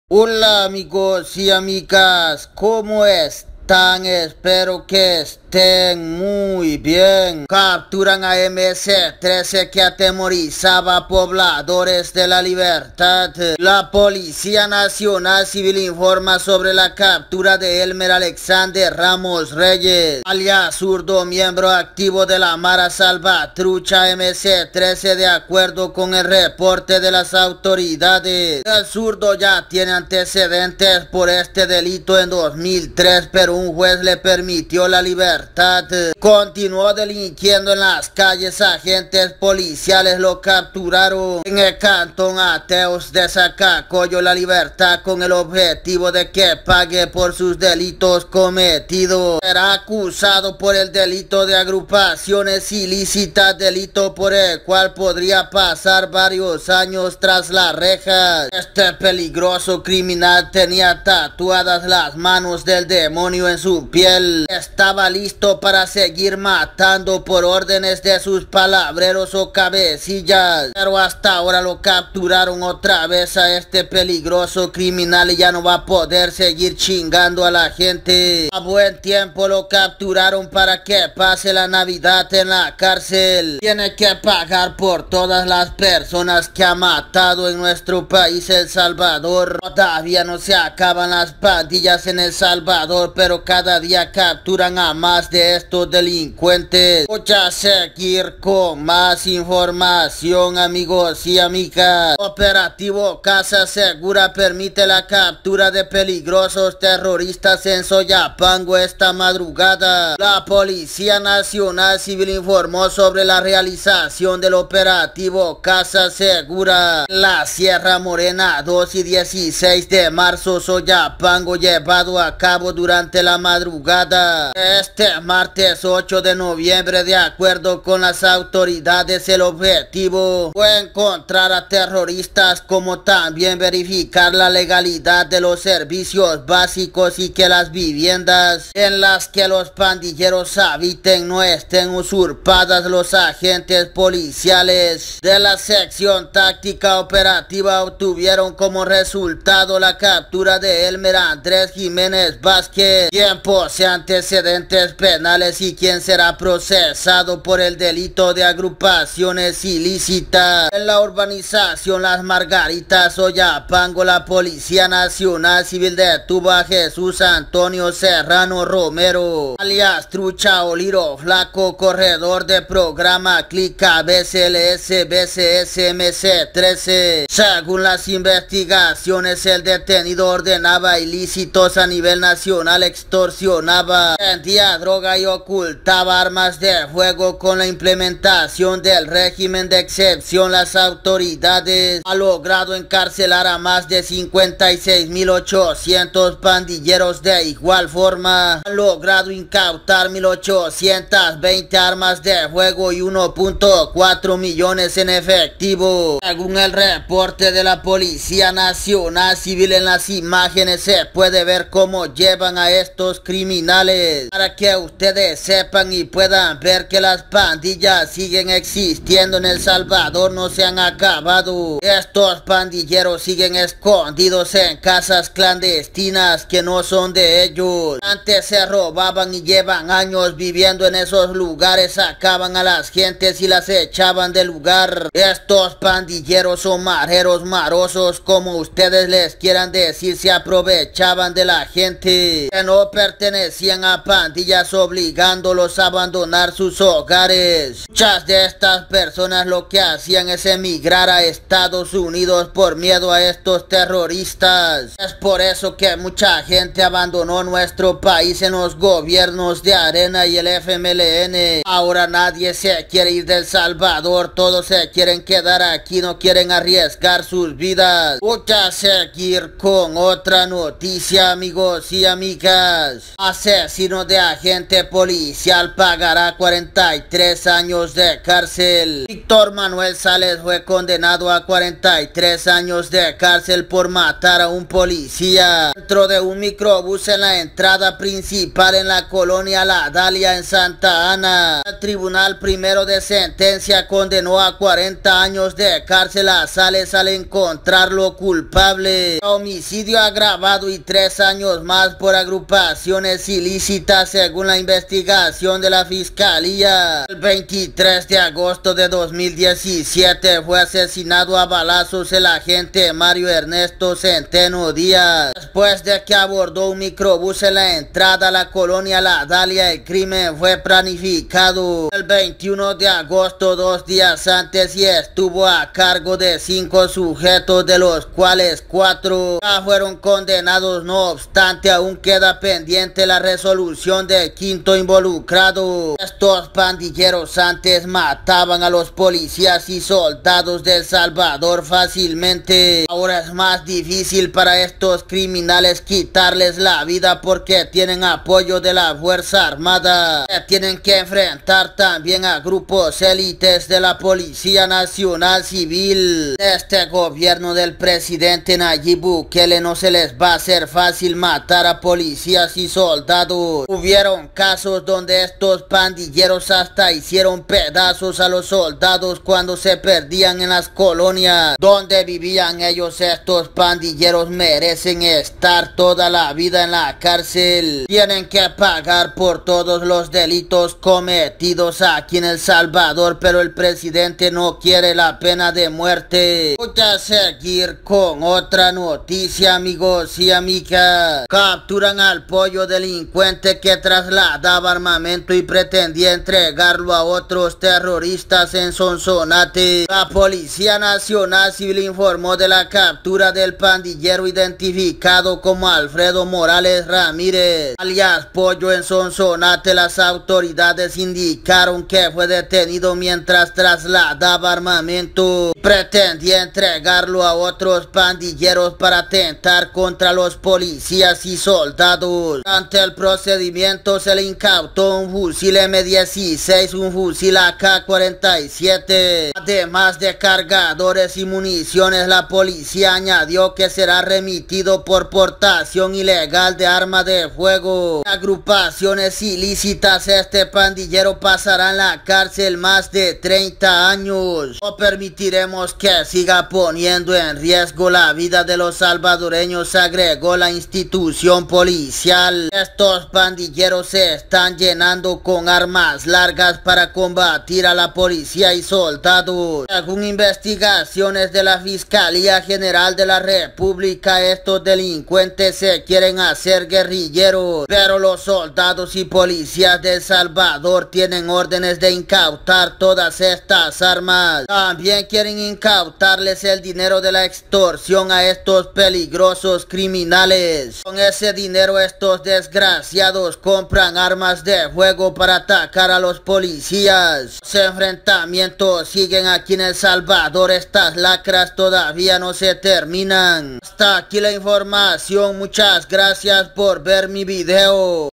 Capturan a MS-13 Operativo Casa Segura padilleros usurpaban viviendas asesinaron a agente policial
delincuente que trasladaba armamento a pandilleros